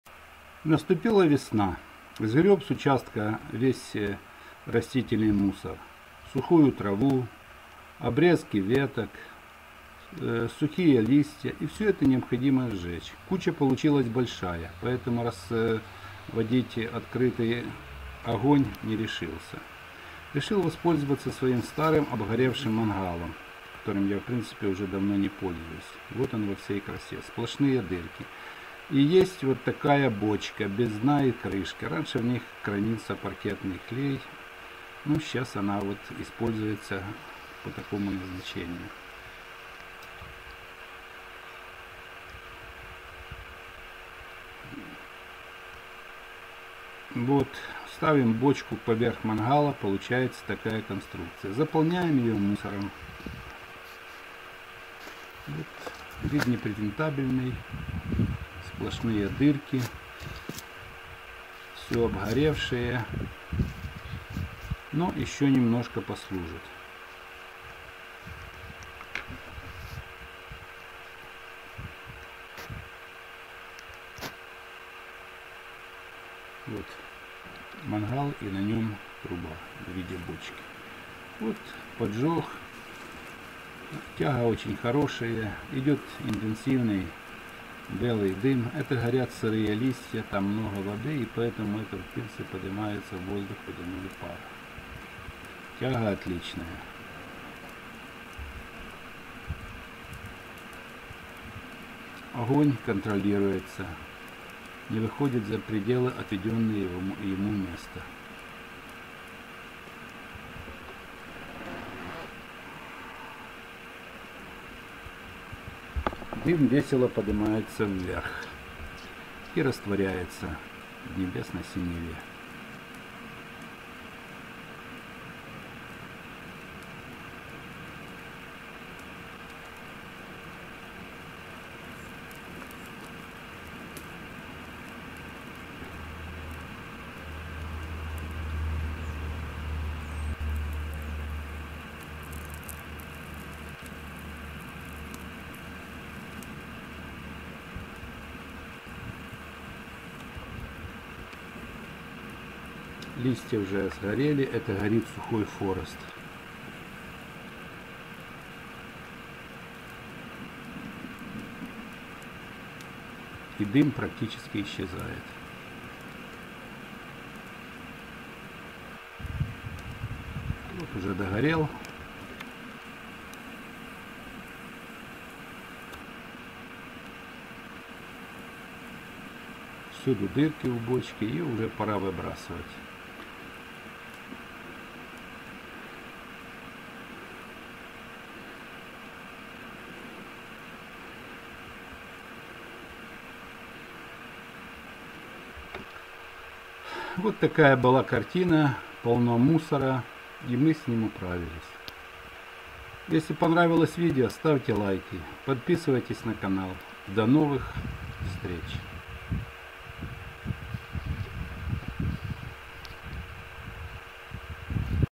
Как сжигать садовый мусор. Печка для сжигания мусора с мангала и бочки.Суперспособ-сжигаем мусор.
Весной приходится сжигать садовый мусор-листья,ветки,траву...Накапливаются горы такого хлама.Чтобы не разводить большой костер использую старый мангал и бочку без дна и крышки..Получается очень эффективная мусоросжигательная конструкция.Для большего эффекта  бросил в бочку несколько еловых веток -пошел густой дым...Ветки быстро сгорели и дым закончился.